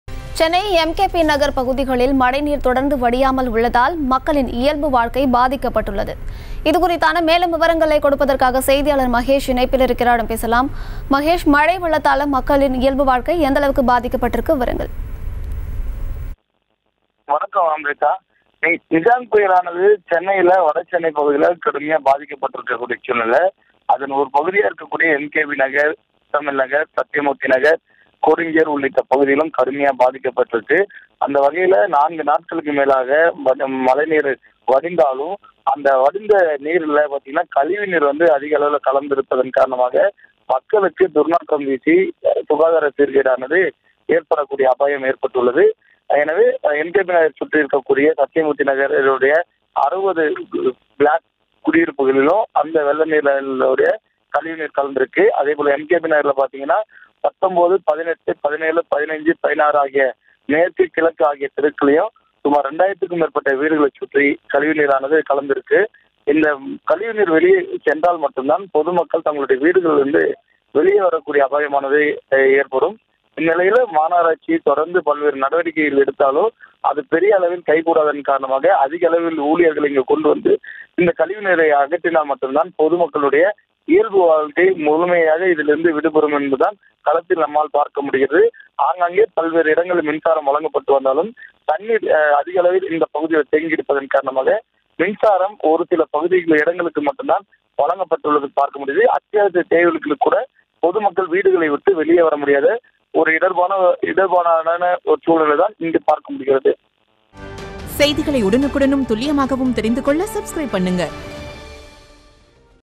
4வது நாளாக மழைநீர் வடியவில்லை.. துர்நாற்றம் வீசுவதாக மக்கள் புகார் | MKB Nagar | Chennai Floods 2023
4வது நாளாக மழைநீர் வடியவில்லை.. துர்நாற்றம் வீசுவதாக மக்கள் புகார் | MKB Nagar | Chennai Floods 2023